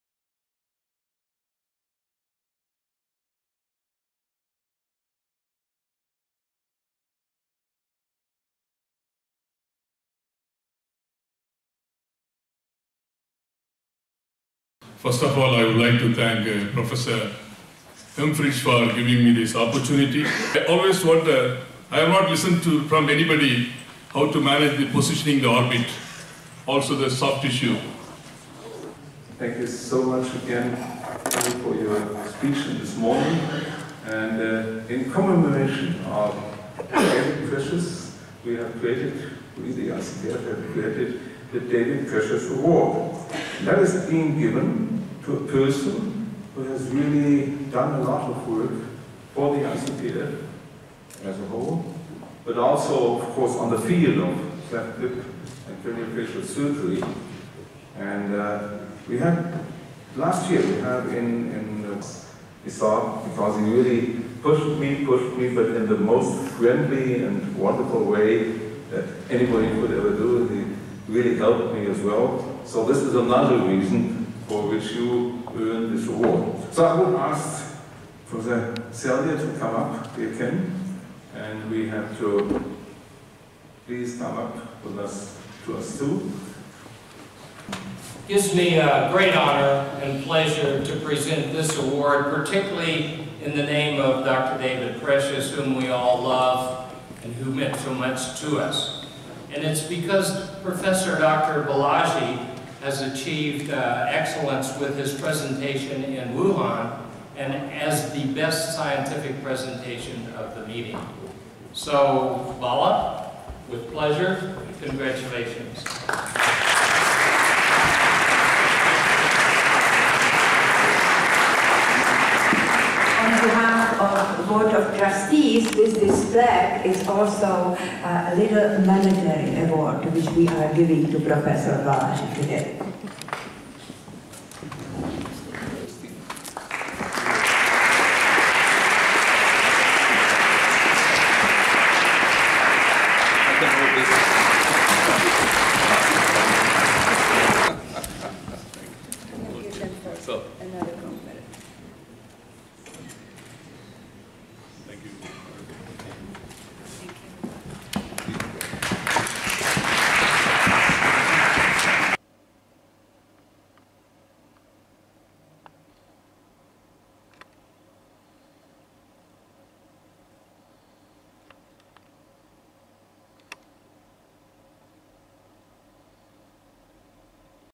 Prof SM Balaji receives the highest scientific award in Germany
Prof SM Balaji has been honored with the highest scientific “Prof David Precious Award”. This was at the 12th World Cleft and Craniofacial Congress held in Leipzig, Germany. He is the first Indian to receive the honor in such a platform. The award comes with a citation, plaque and a cash award of US $1000.00. Prof Kenneth Salyer, world renowned Craniomaxillofacial surgeon, presented him with the award. He is the President of the International Cleft Lip and Palate Foundation. Congress President, Prof Alexander Hemprich was also present. Prof Marie Tolarova, Member, Board of Trustees, ICPF spoke a few words about Dr S M Balaji's achievement. This award is the highest award of the International Cleft Lip and Palate Foundation.

Achievements of International Cleft Lip and Palate Foundation
The International Cleft Lip and Palate foundation is a thirty year old forum. It is multidisciplinary with members including all stakeholders involved in orofacial cleft rehabilitation. This is a non-profit organization. It promotes service to humanity and dissemination of scientific knowledge. The ICPF coordinates volunteer surgical missions in underdeveloped and developing countries. It sponsors surgeries in Vietnam, Cambodia, Tunisia and various other developing countries. They teach the local surgeons and cleft teams about cleft rehabilitation. The ICPF has its headquarters in Japan and has over 3000 members from over 60 nations across the world.

Prof David Precious, Cleft Lip and Palate Specialist
One of the founding Presidents of ICPF was Prof David S Precious. Prof David Precious resided, taught and served in Halifax, Canada. He was a notable Cleft surgeon who trained under Prof Delaire. Prof Delaire was the founding father of Craniomaxillofacial and Plastic Surgery. Dr. Precious established a Cleft Surgery unit in Canada. Prof Precious also continued research into Cleft treatment. He had many publications to his credit. In 2015, Prof David Precious passed away after a brief illness. The ICPF decided to instate a Scientific Award in his honor. It was in recognition of his immense scientific contributions to Orofacial Cleft rehabilitation. This is the highest Scientific Award of this noted International Organization.

International Recognition for Prof S M Balaji’s Ground Breaking Research
Prof SM Balaji conducts research in Protein Guided Tissue Regeneration. He uses this for surgical cleft correction. The award is for these groundbreaking studies. Scientific merit was the basis of selection for this award. The selection panel consisted of world renowned cleft rehabilitation surgeons. Prof S M Balaji’s pioneering work removed the necessity of a second surgery and more bone grafts. This led to great improvement in the quality of life of the patients. It also avoided a lot of complications associated with synthetic substances.